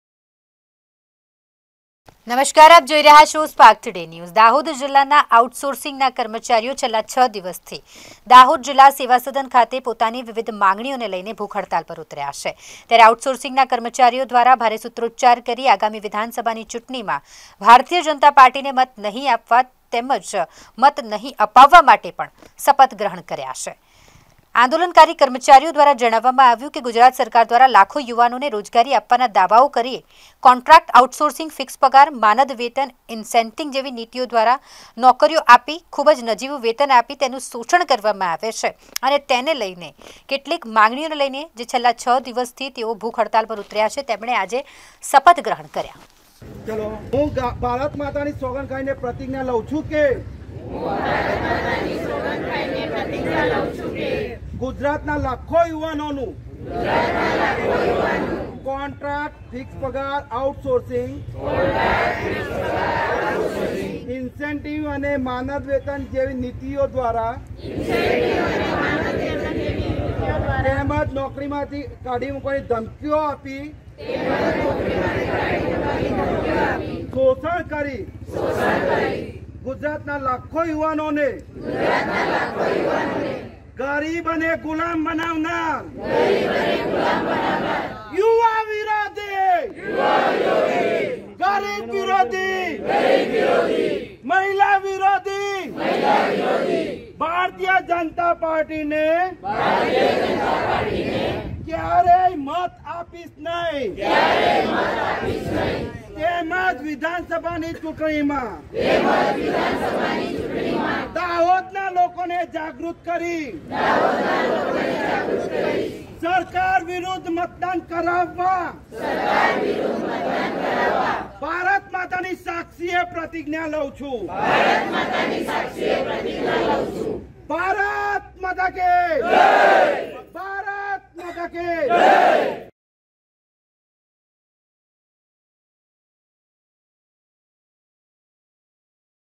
દાહોદ : આઉટસોર્સિંગ કર્મચારીઓએ ભારતીય જનતા પાર્ટીને મત નહીં આપવા શપથ લીધા2022 | Spark Today News
દાહોદ : આઉટસોર્સિંગ કર્મચારીઓએ ભારતીય જનતા પાર્ટીને મત નહીં આપવા શપથ લીધા